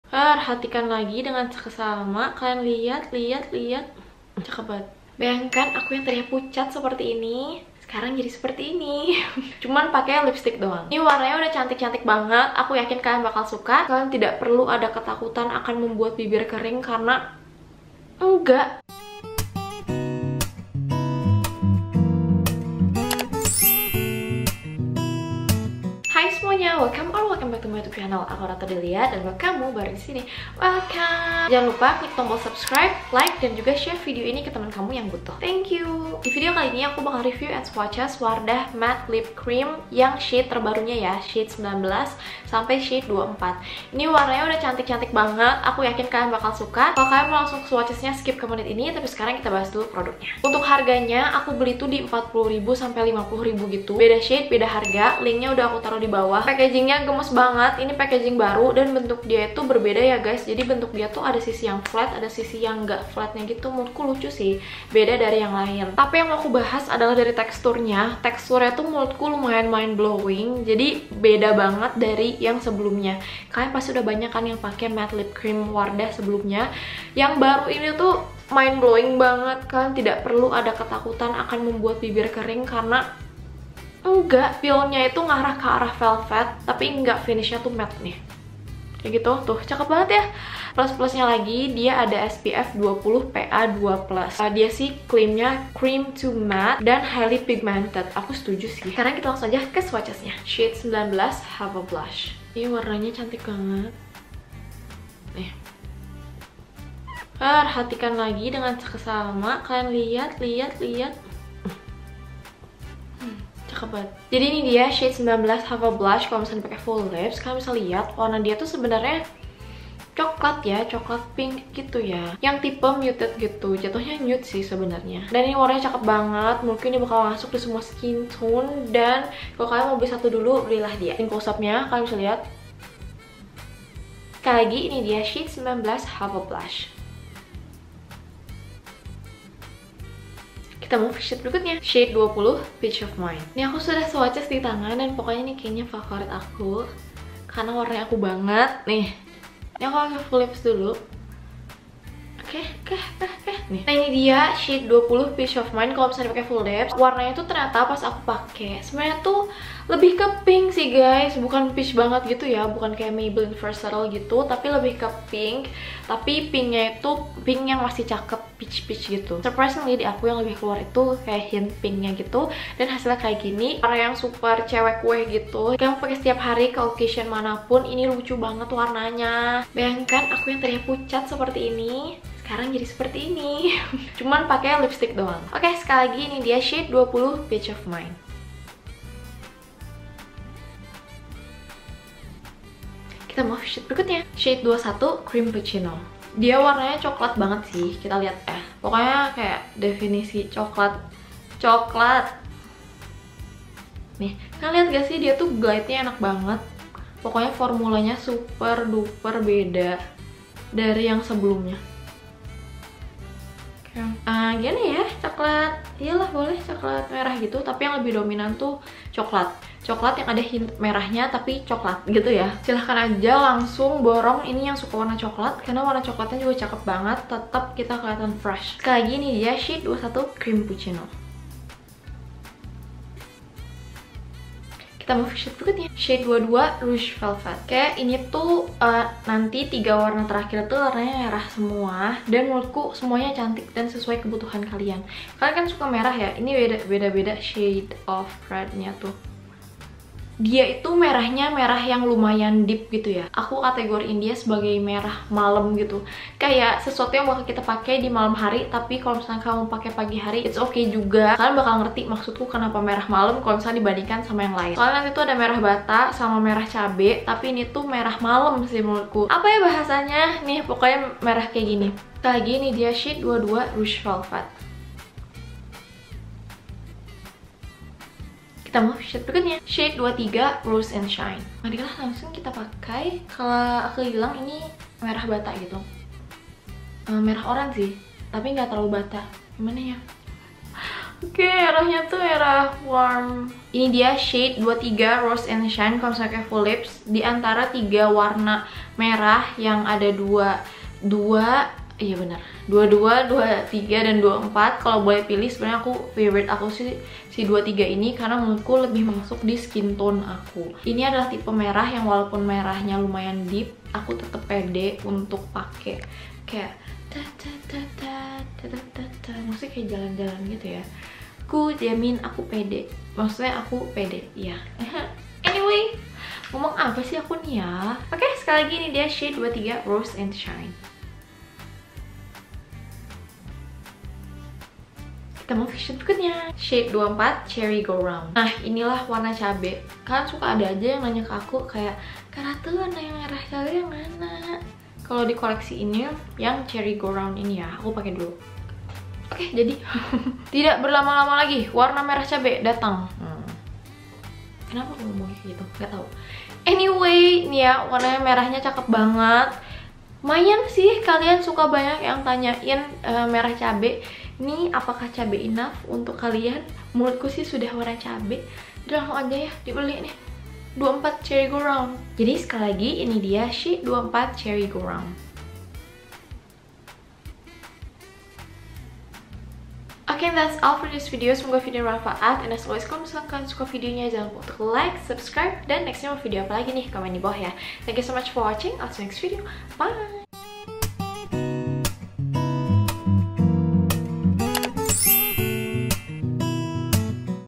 WARDAH MATTE LIP CREAM TERBARU 19-24 Ratu Adellya
//timestamps//
00:00 teaser
01:40 Shade 19 Have a Blush!
02:21 Shade 20 Peach of Mind
03:14 Shade 21 Creampuccino
03:44 Shade 22 Rouge Velvet
04:39 Shade 23 Rose and Shine
05:36 Shade 24 Cherry Go Round

//equipments//
- lighting : matahari (no filter, no additional lighting)
- app : imovie